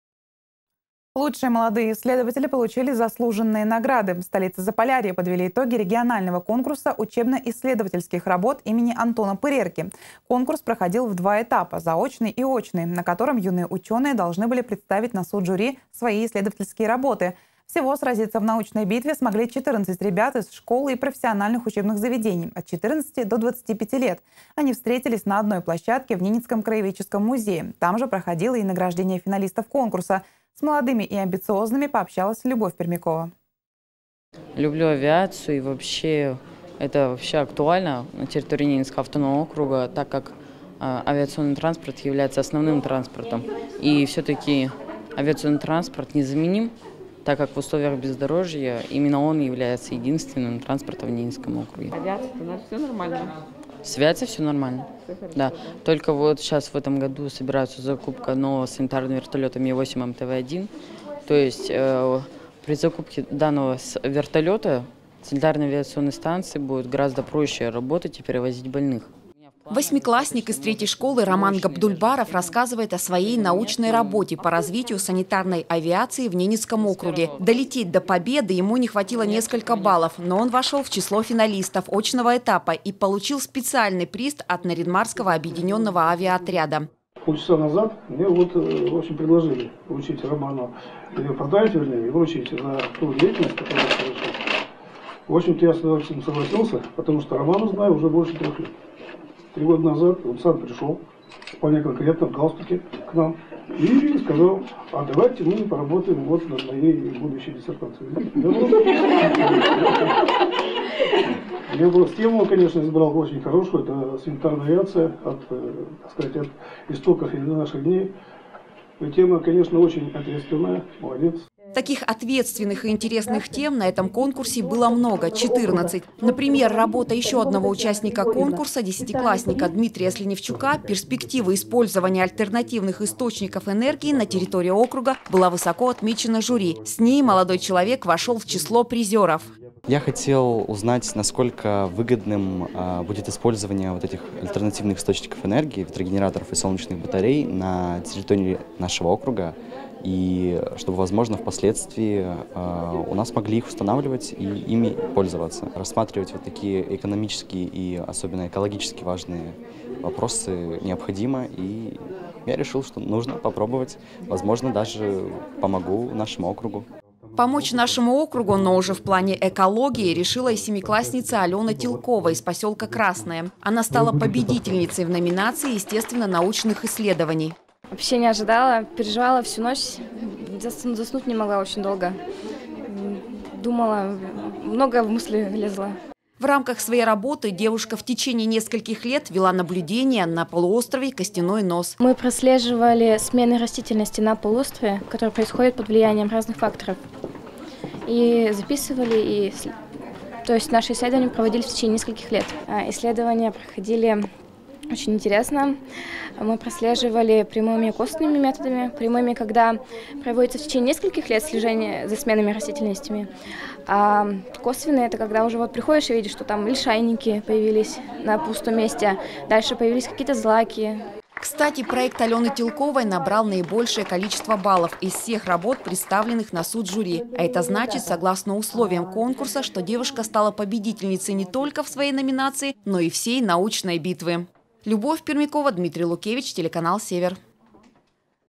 В Нарьян-Маре подвели итоги конкурса учебно-исследовательских работ имени Антона Пырерки
Лучшие молодые исследователи получили заслуженные награды. В столице Заполярья подвели итоги регионального конкурса учебно-исследовательских работ имени Антона Пырерки. Конкурс проходил в два этапа — заочный и очный, в которых юные ученые должны были представит на суд жюри свои исследовательские работы. Всего сразиться в научной битве смогли 14 ребят из школ и профессиональных учебных заведений от 14 до 25 лет. Они встретились на одной площадке в Ненецком краеведческом музее, там же проходило и награждение финалистов конкурса. С молодыми и амбициозными пообщалась Любовь Пермякова.